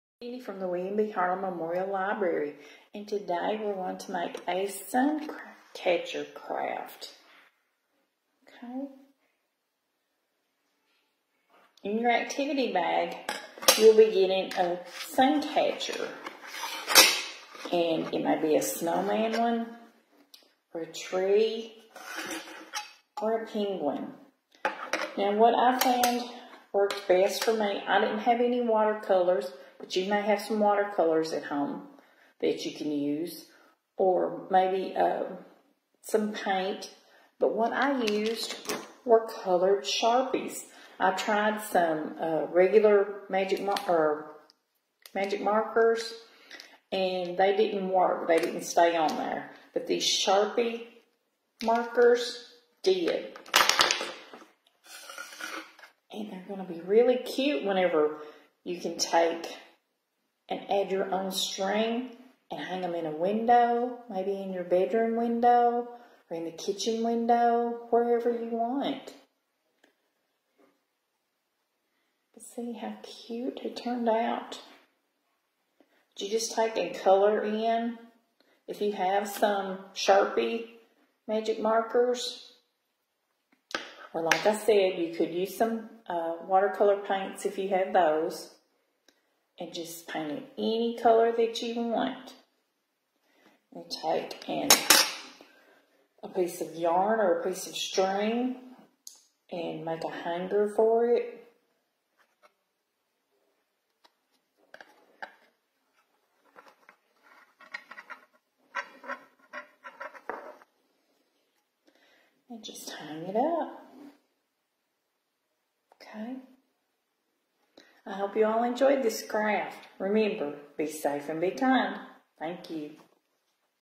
Story Time Crafts with WBHML Sun Catcher Snow Globe with Ms Cindy
On this episode of Story Time Crafts with WBHML, Ms. Cindy has a Sun Catcher Snow Globe craft inspired by the book "Pinkalicious and the Snow Globe" written and illustrated by Victoria Kann.

Materials:
• Sun Catcher Snow Globe (in your January 3rd, 2022 Activity Bag)
• Watercolors, Paint, or Sharpies
• Ribbon or String for hanging